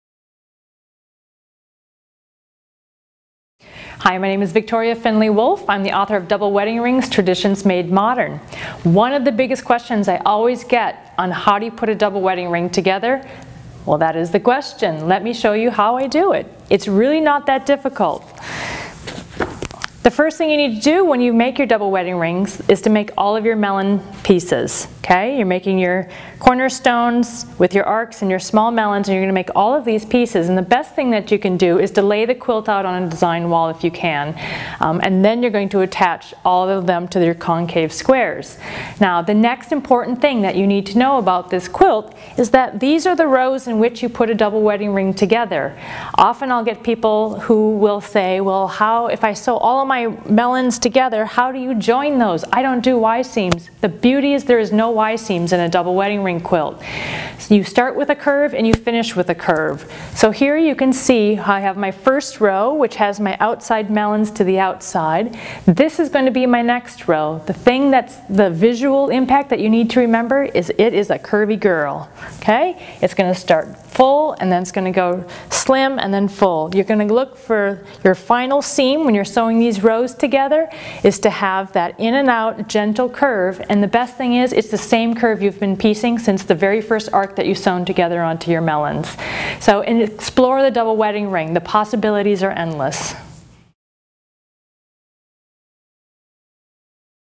Double Wedding Ring Quilts: Piecing the Strips
Get ready for new adventures in conventional piecing with celebrated quilter Victoria Findlay Wolfe. Create stunning Double Wedding Ring quilts with breathtaking innovations on the classic pattern. With full-size patterns for 10 quilts, the book will teach you the Double Wedding Ring basics. After you’ve mastered curved foundation piecing, try your hand at Victoria’s unique fabric slashing and “Made-Fabric” methods—it’s easier than you think! You’ll feel liberated as you improvise on her designs, with full instruction for some quilts and others that invite your creative discovery. Read the stories that inspired each of Victoria’s designs, and then take inspiration from the artist at work in her studio, with photography of her creative process and 3 bonus quilts to jumpstart your own art.

• 13 modern Double Wedding Ring quilts and their stories
• Peek inside the studio of award-winning quilter Victoria Findlay Wolfe
• Full-size patterns! Learn paper piecing, fabric slashing, and tips to create “Made-Fabric”
• Modern traditional designs with fresh color choices
• From the award-winning author of 15 Minutes of Play—Improvisational Quilts